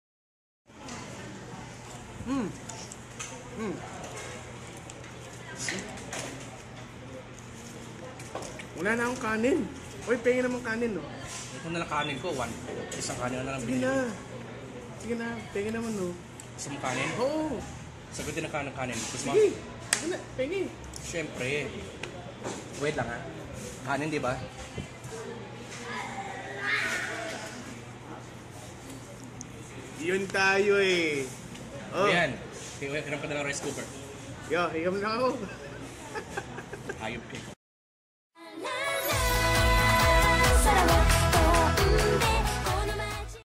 No rice? no problem! WALA NA KANIN!!!
It seems when you're enjoying your meal and you realized you ran out of rice atleast there's a way